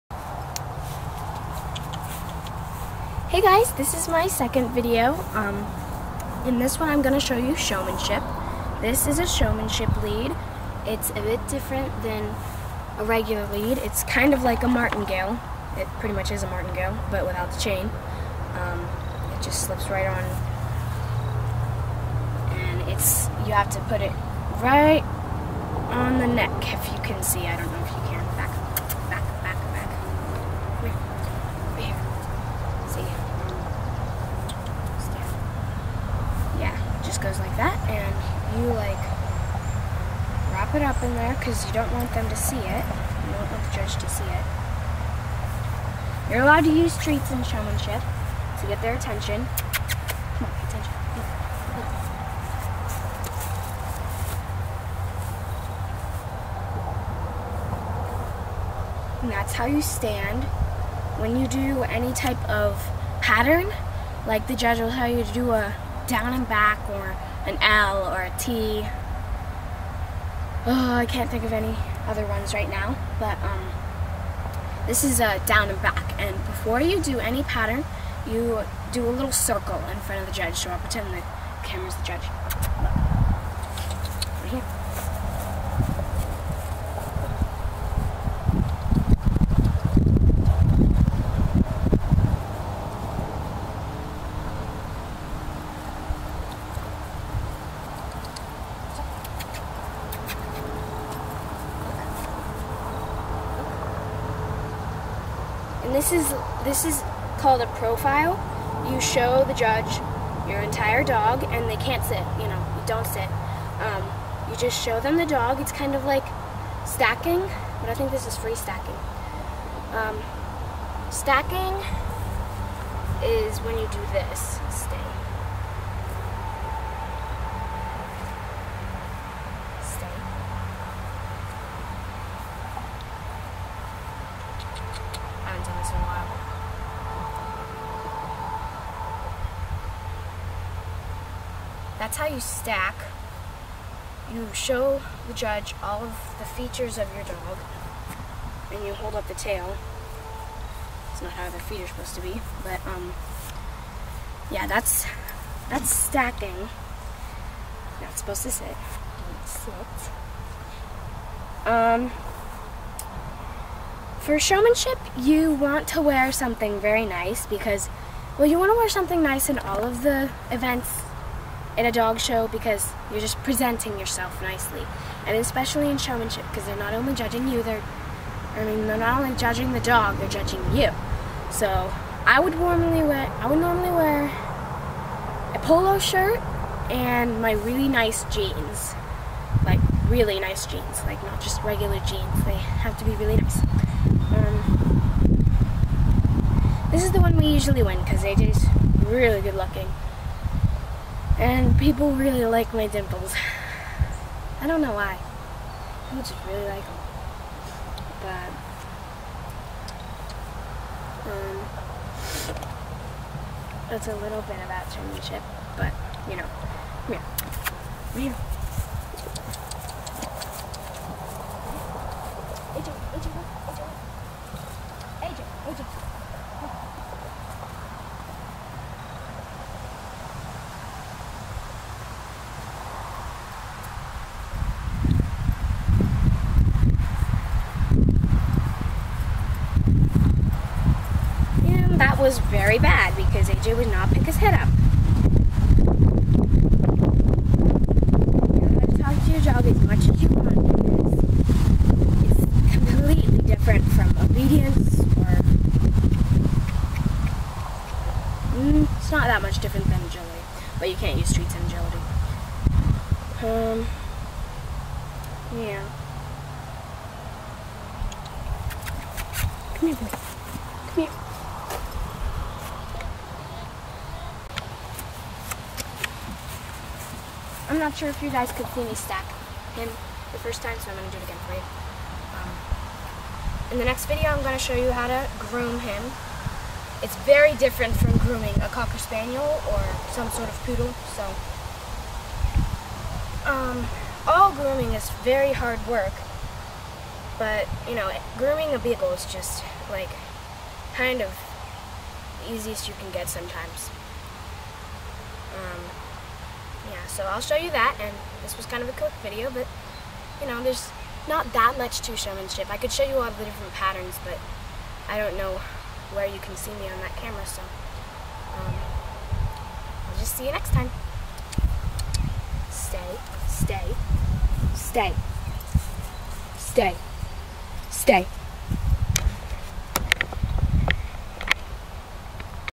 Dog Trainer Series - Part 2 - Showmanship
This isn't the best representation of showmanship, but I may post a better one later. I might post some footage of me in one of my shows coming up of me in showmanship.  In the next video I will show you how I groom my beagles.